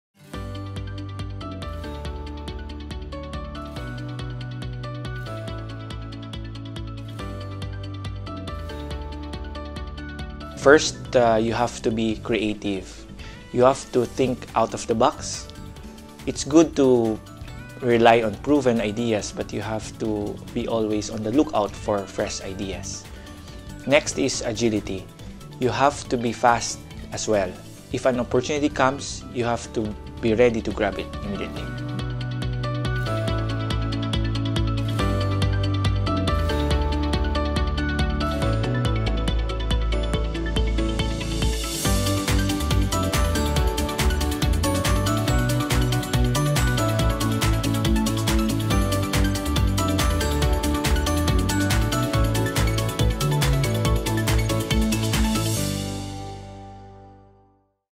16th Mansmith YMMA - Search is On!
The MANSMITH YOUNG MARKET MASTERS AWARDS (Mansmith YMMA) is one of Mansmith and Fielders, Inc.’s advocacies, aimed at providing INSPIRATION by recognizing excellent Filipino marketers, sales managers and entrepreneurs as today’s role models.

This project seeks to honor young achievers, 35 years old and below, who excel in marketing, sales and entrepreneurship, and whose achievements encourage and inspire young people to be innovative entrepreneurs and outstanding brand builders.

Or download the application form

* There are no entry fees to join.
* Judges in the Mansmith YMMA are chosen from non-competing companies (only after entries are shortlisted) to ensure impartial decision.